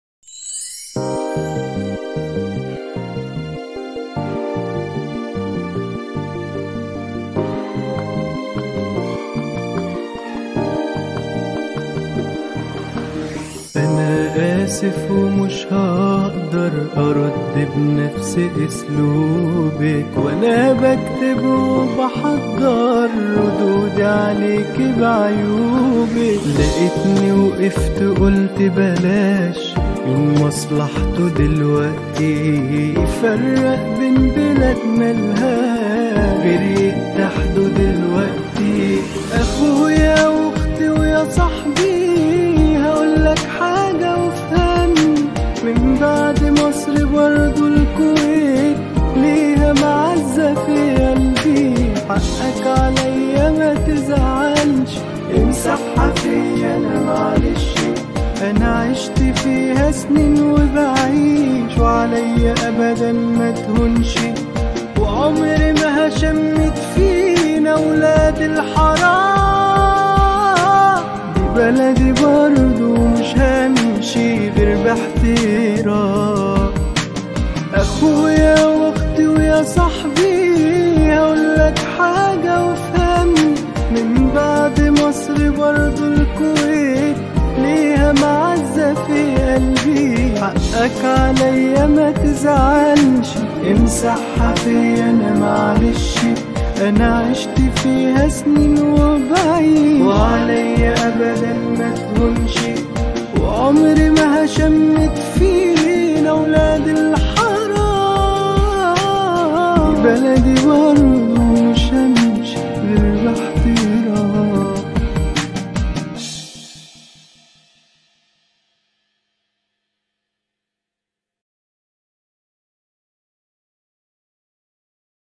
August 13, 2020
اقوي اغنية رد علي
اقوي رد من مصري بالكويت  علي ريم الشمري
مصر و الكويت اخوات
لا لفتنة بين بلدين اشقاء